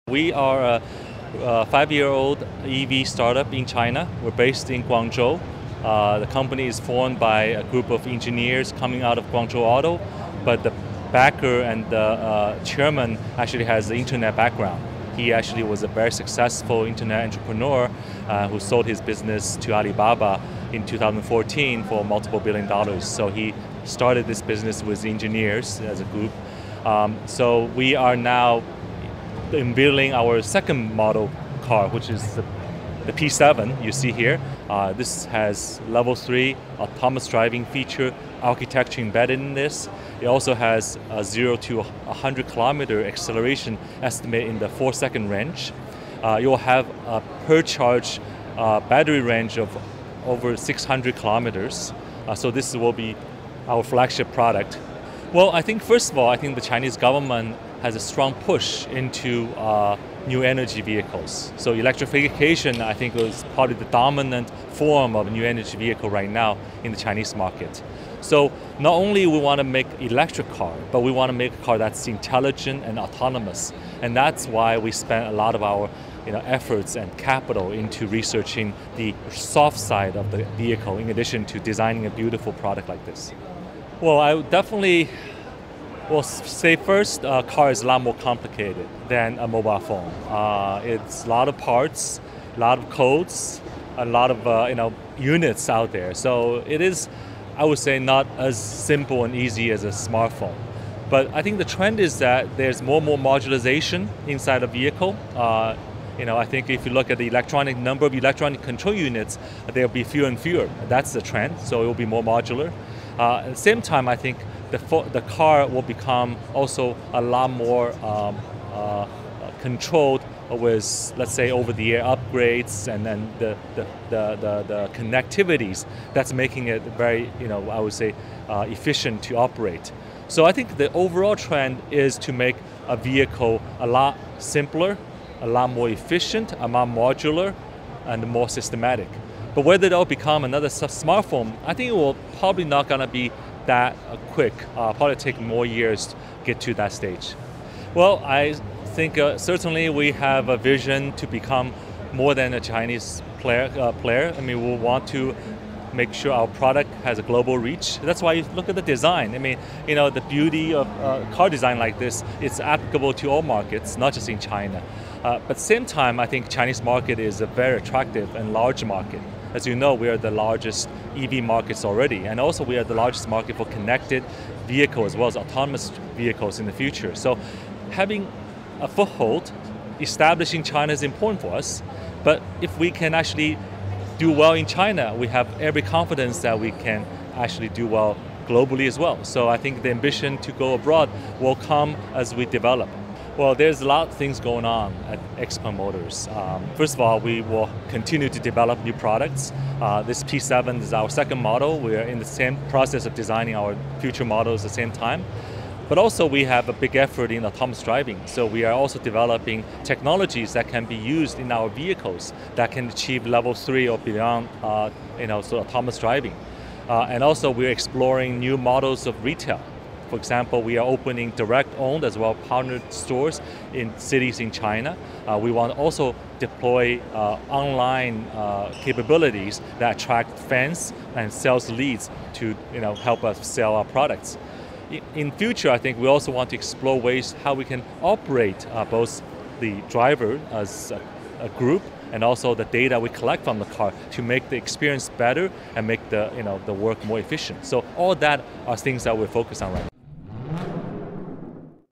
Electric Xpeng P7 Tesla Rival
Xpeng P7 specs:

600 km (373 miles) of NEDC range
dual-motor all-wheel drive
0-100 km/h (62 mph) in 4 seconds
Length of 4.9m
Wheelbase 3m
ready for Level 3 autonomous driving